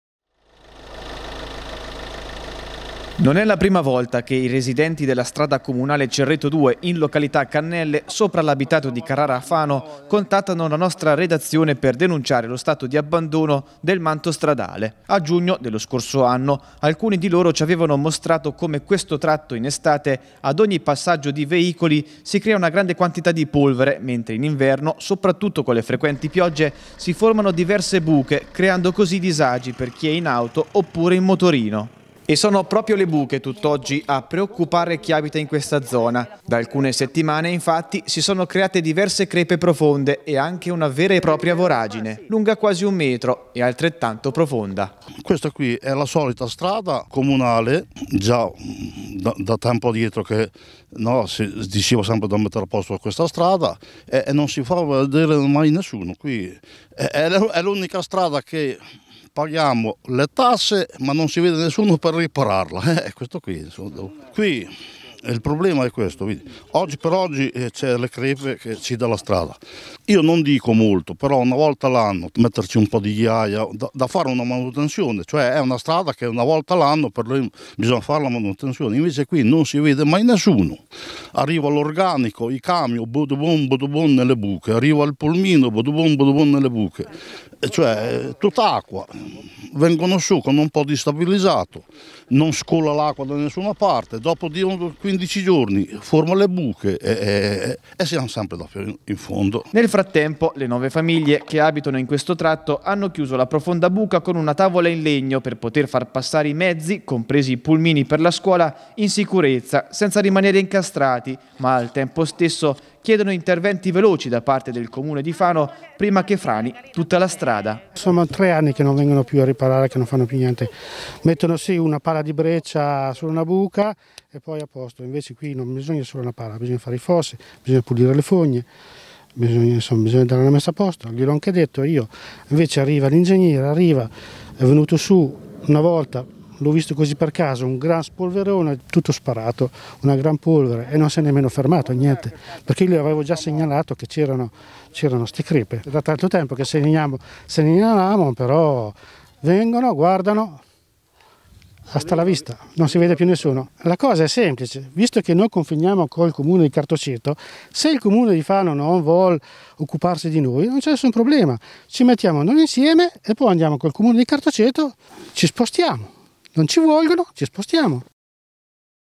Voragine preoccupa i residenti di località Cannelle
Una voragine si è creata nei giorni scorsi in una strada che collega 9 nuclei familiari. È successo in località Cannelle, sopra il centro abitato di Carrara. I residenti chiedono interventi prima che il tratto stradale ceda completamente.